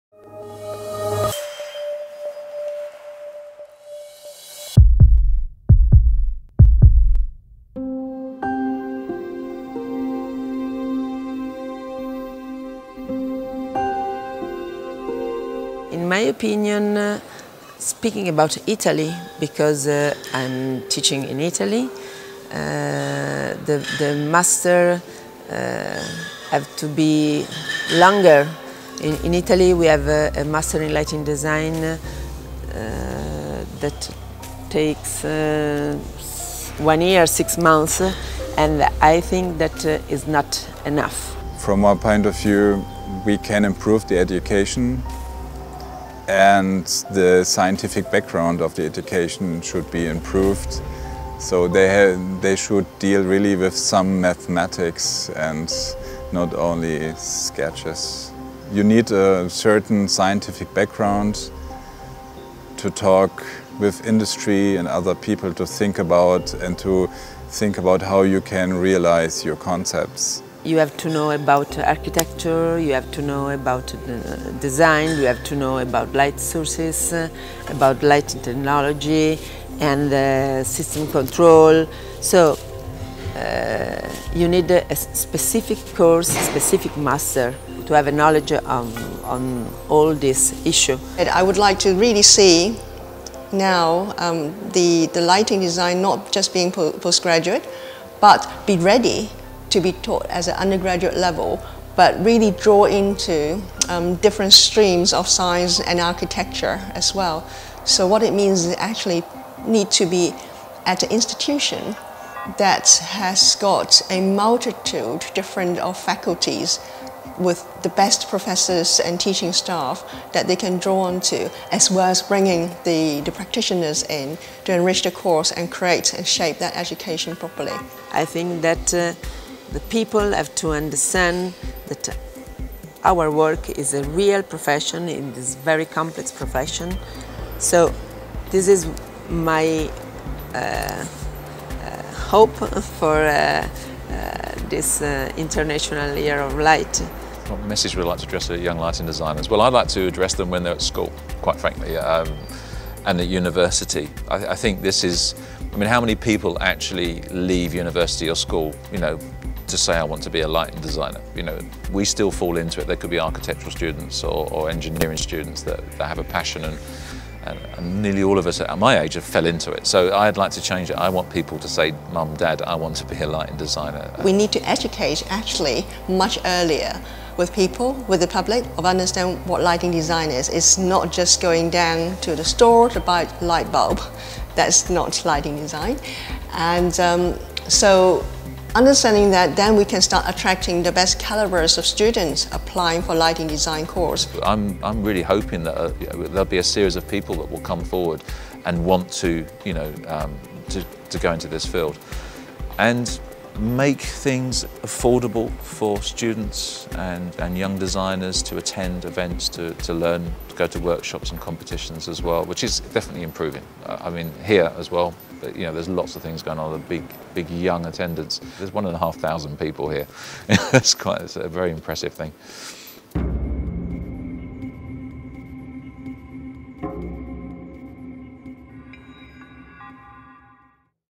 How to Design with Light using Education?
Interviews with renowned lighting designers: Raoul Hesse, Florence Lam, Juliette Nielsen, Marinella Patteta and Martin Valentine.

Questions
• How do you take part to education in lighting design?
• What is your vision of the future of light and education?

At the Professional Lighting Design Convention (PLDC) 2015 in Rome, Italy, Philips Lighting spoke to recognized lighting designers.

Our thanks go to the Ergife Palace Hotel, restored by the studio of architecture, Lorenzo Bellini Associates – LBA – which hosted these conversations.